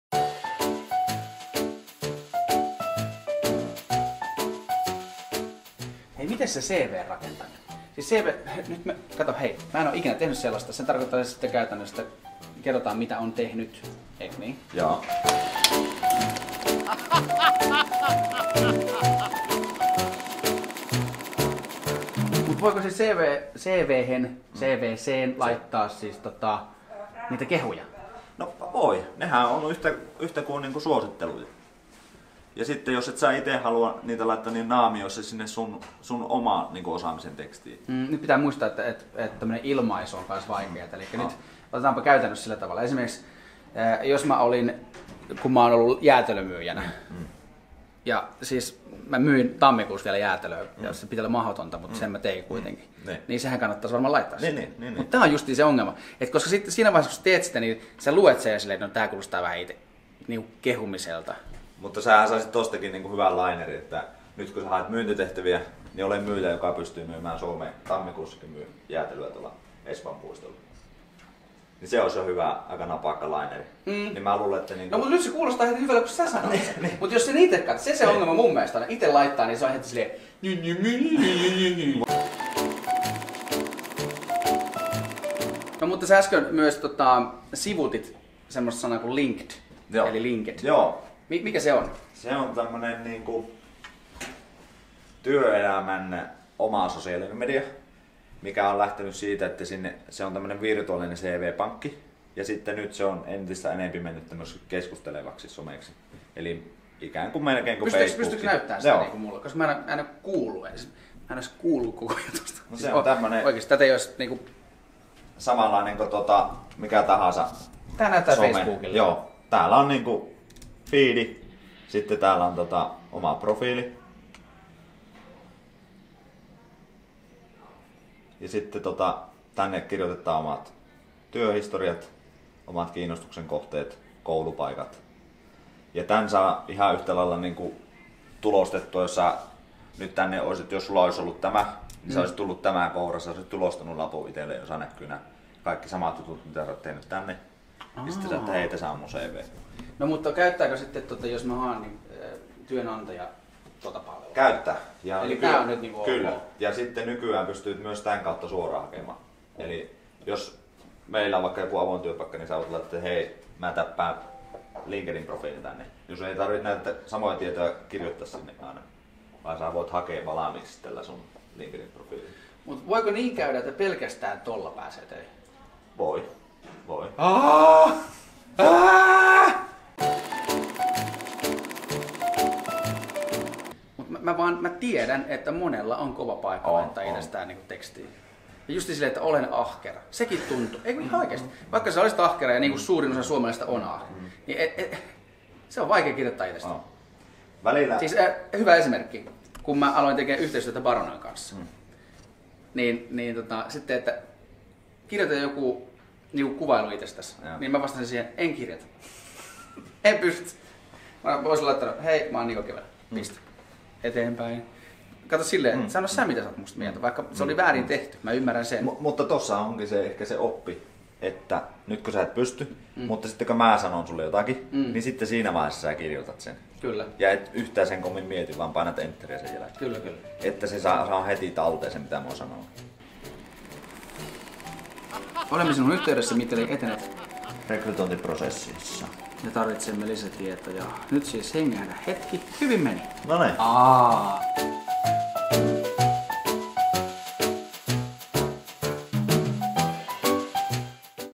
Barona & Niko Kivelä - työnhakuvinkit: Somepresenssi ja CV
JAKSO 1. Niko Kivelä jututtaa Baronalaisia, miten tehdään hyvä CV, miten töitä haetaan, miten haastatteluun kannattaa valmistautua ja kuinka lopulta haastattelussa saadaan itsestä se parhain irti. Tsekkaa työpaikat careers.barona.fi